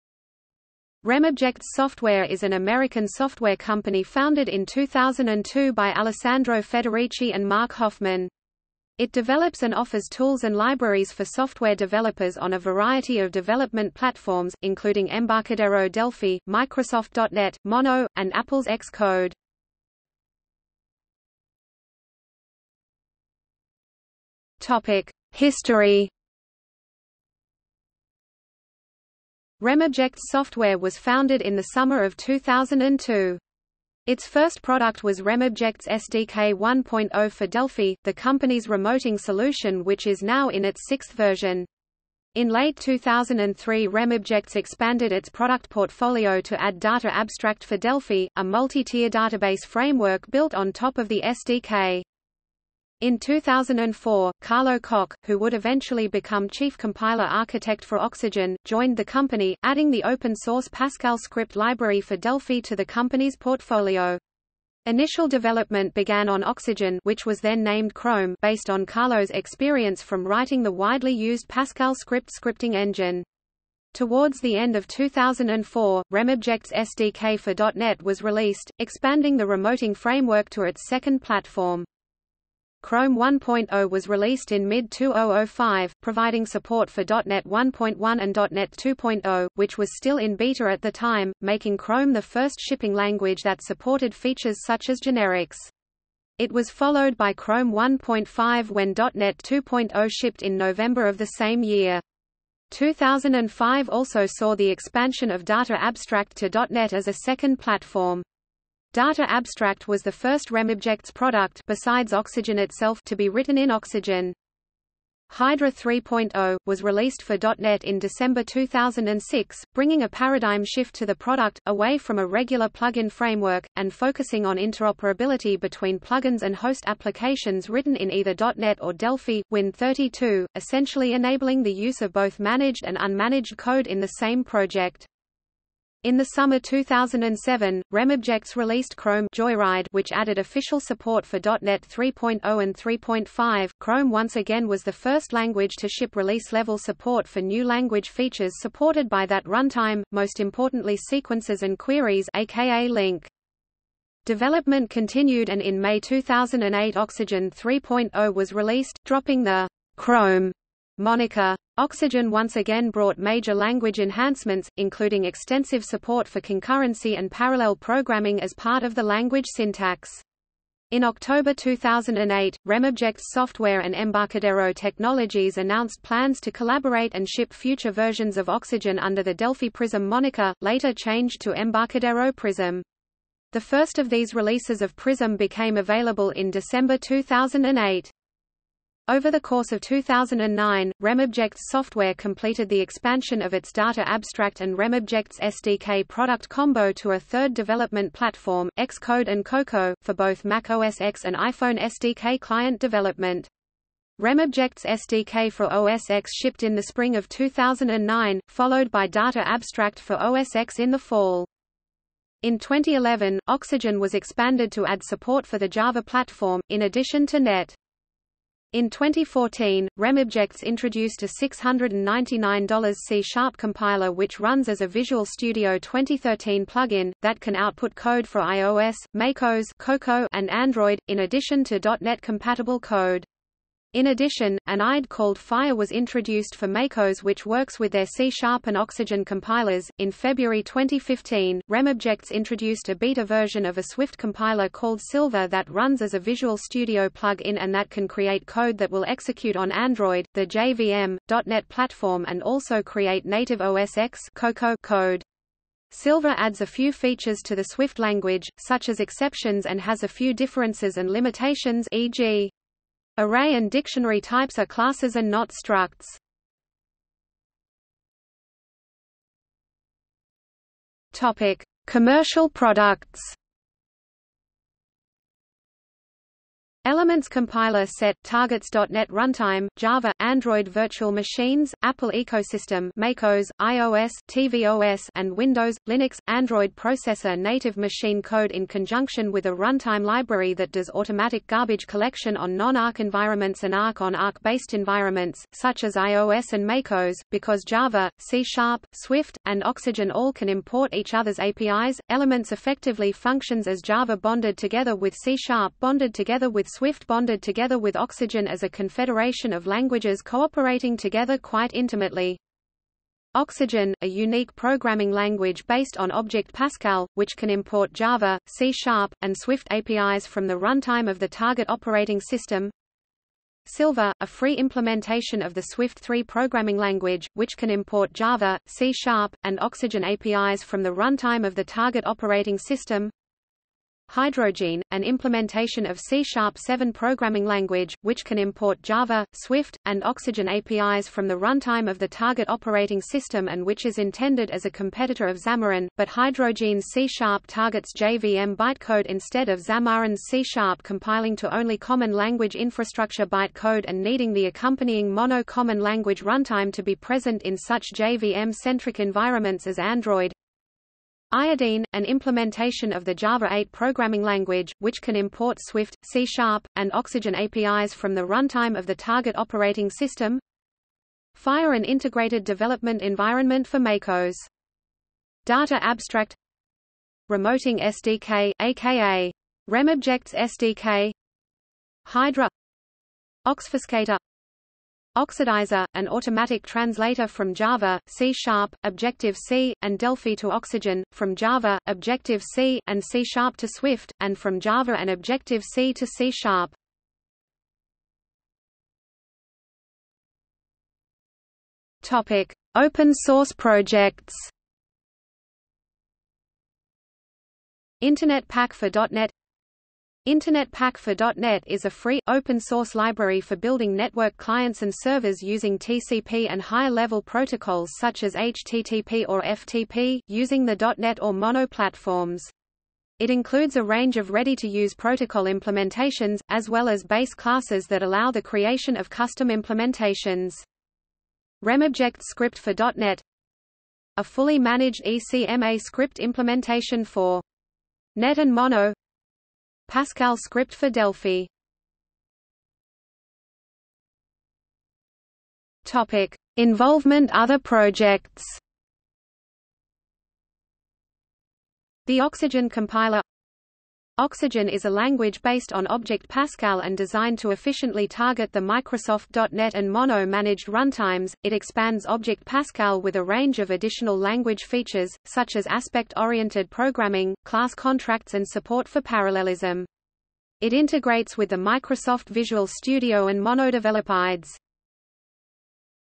RemObjects Software | Wikipedia audio article
00:00:21 1 History
00:04:27 2 Commercial Products
00:06:38 3 Open Source Projects
00:07:22 4 Involvement Other Projects

- Socrates

SUMMARY
RemObjects Software is an American software company founded in 2002 by Alessandro Federici and Marc Hoffman. It develops and offers tools and libraries for software developers on a variety of development platforms, including Embarcadero Delphi, Microsoft .NET, Mono, and Apple's Xcode.